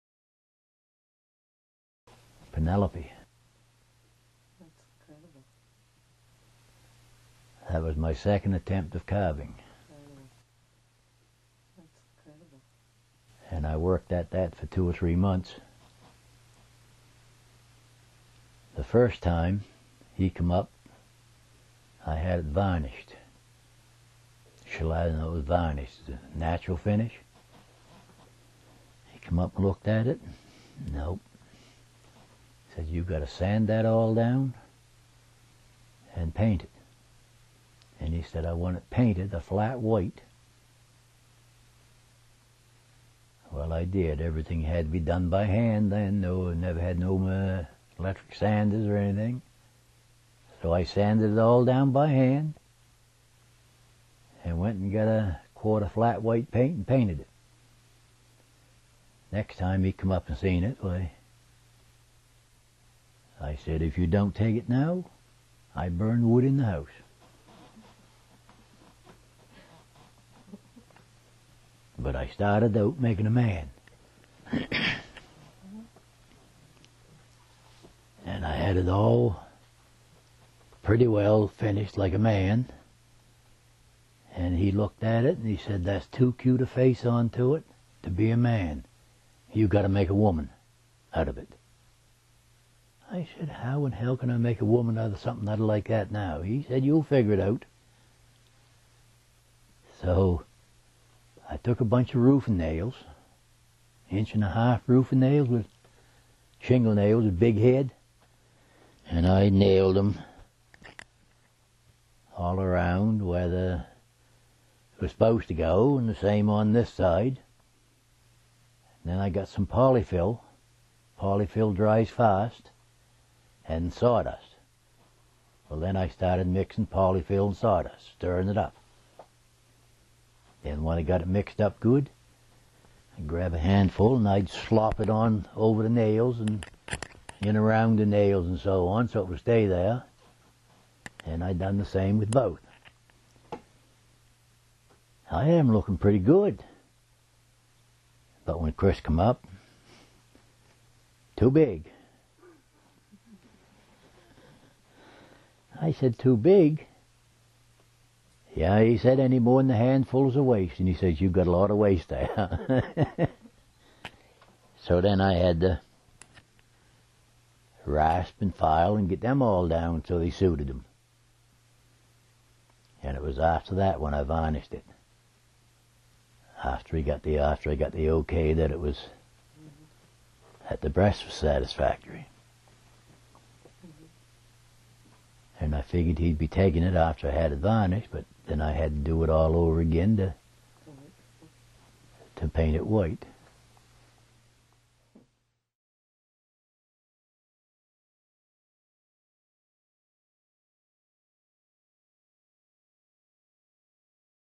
Bubby Mooers - Penelope
Folk Artist Clarence (Bubby) Mooers tells the story of the creation of Penelope, a carving which is now in the permanent collection of the Canadian Museum of Civilization.